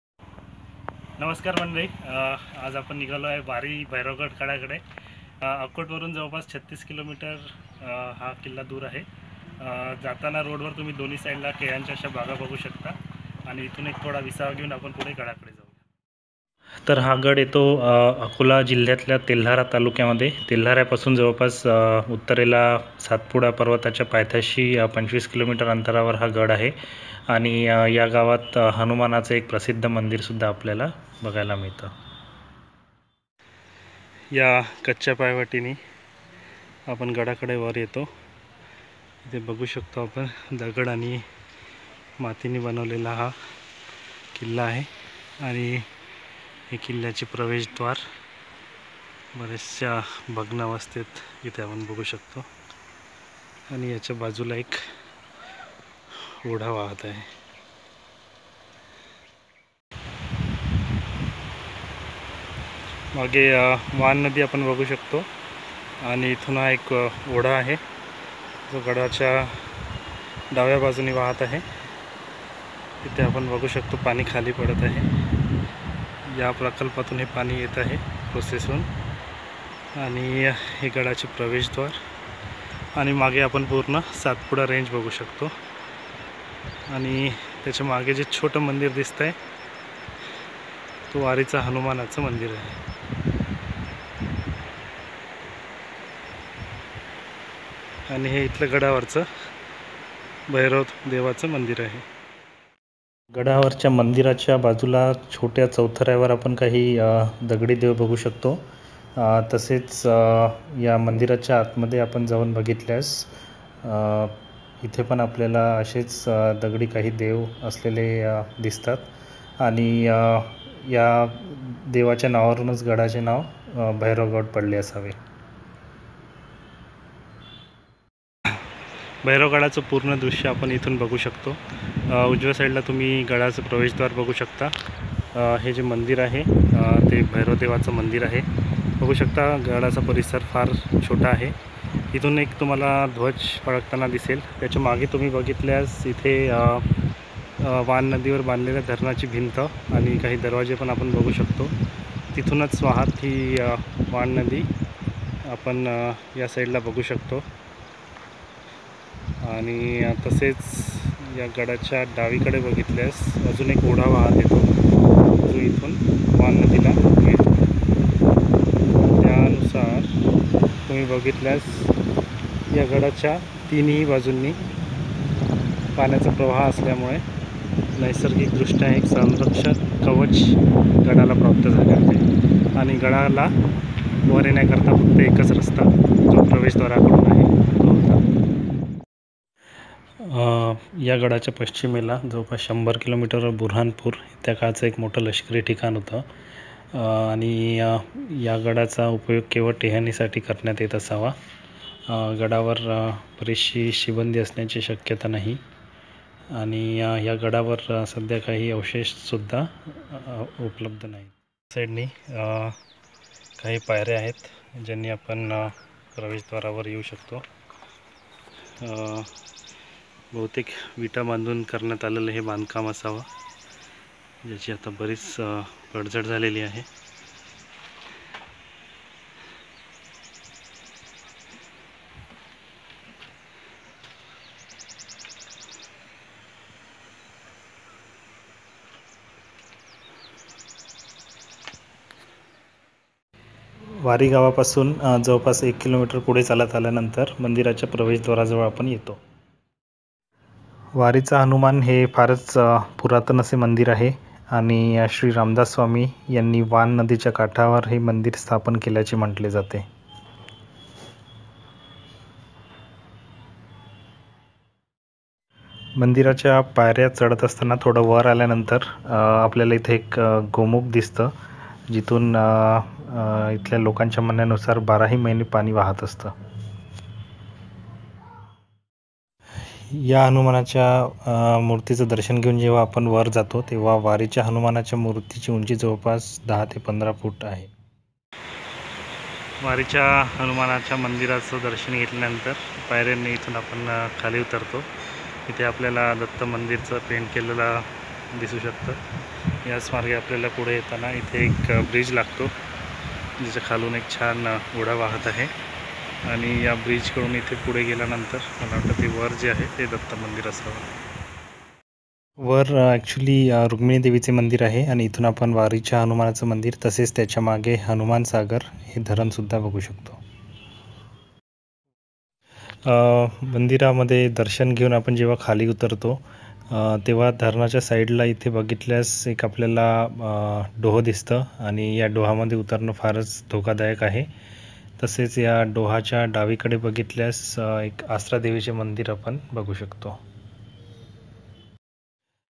Wari Bhairaogad Fort, Telhara, Akola | वारी भैरवगड, तेल्हारा, अकोला
Small Hill fort on the bank of Wan river. There is also an ancient temple of Lord Hanuman built by Shri Ramdas Swami.